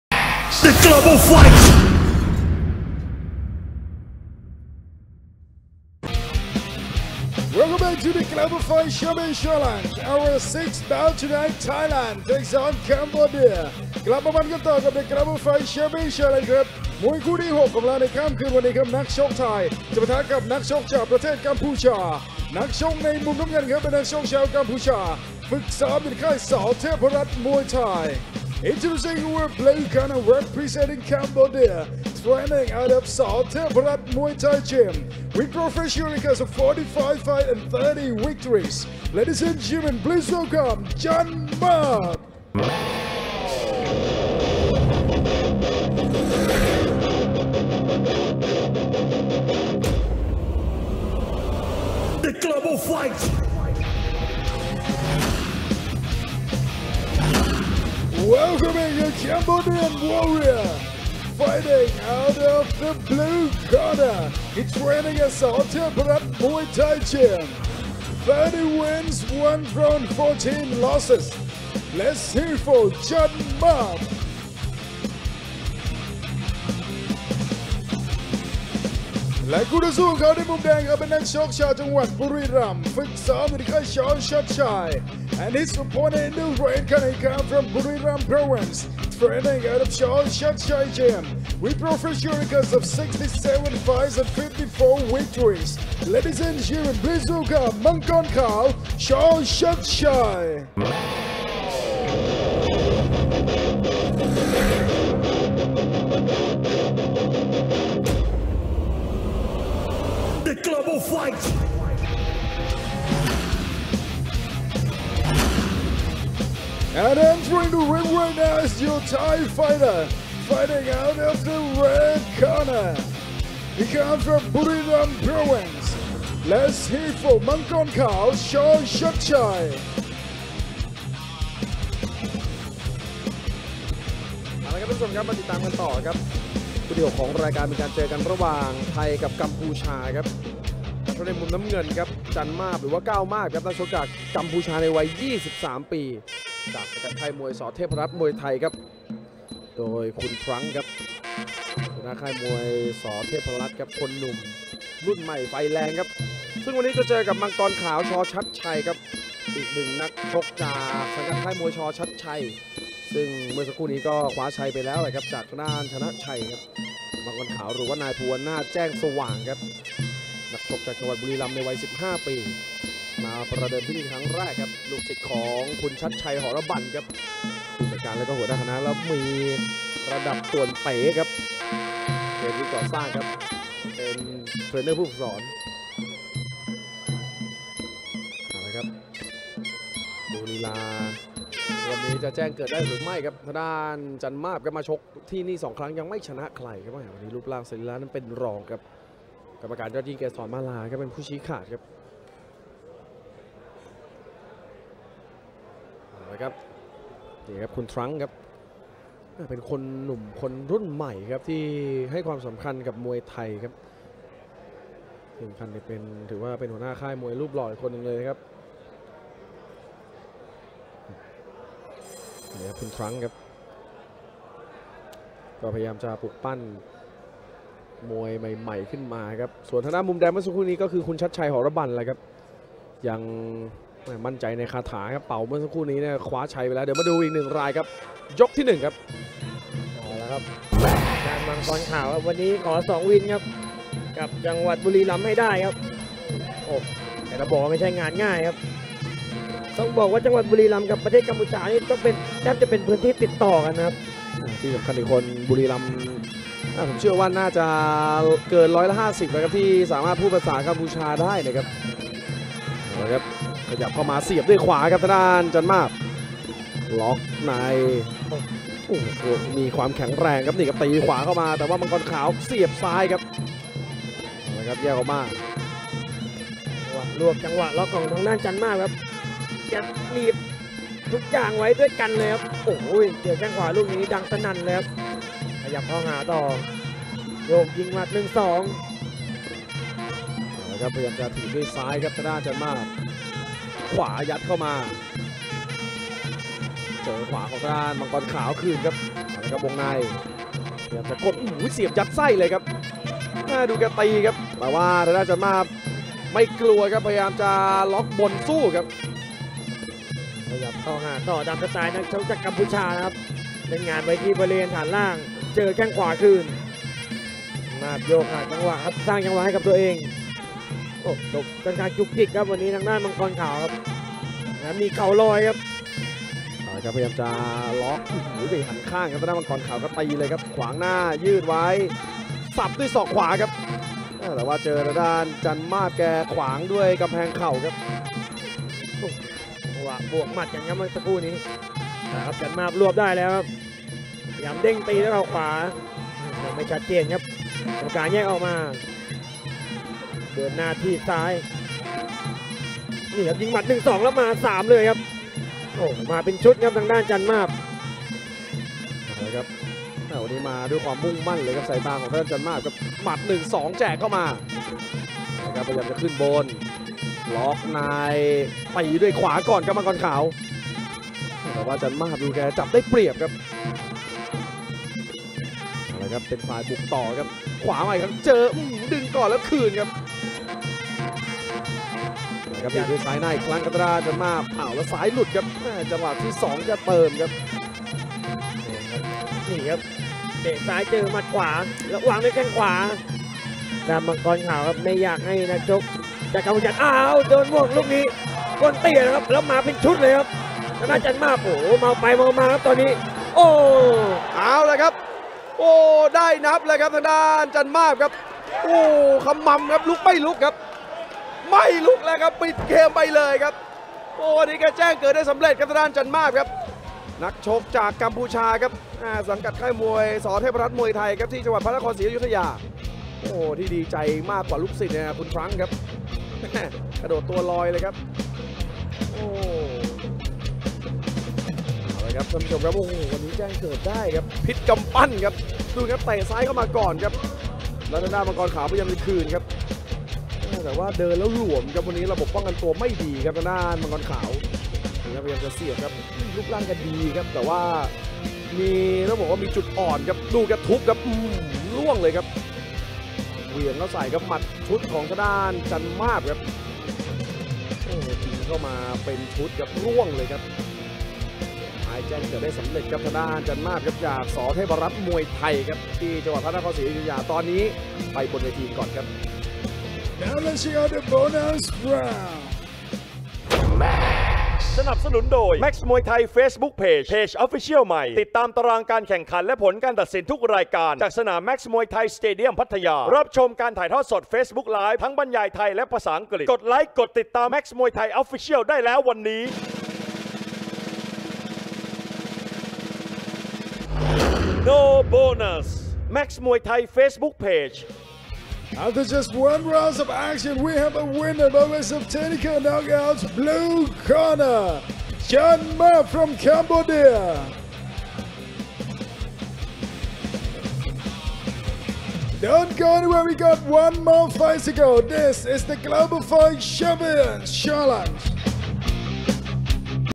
Max Fight 2019: Kao Mab (Cambodia) vs Mangkorkaw Chor.chatchai (Thailand)
Full fight:  Kao Mab (Cambodia) vs Mangkorkaw Chor.chatchai (Thailand)
Max Muay Thai 12/19/2019

🔵 ATTRIBUTION 🔵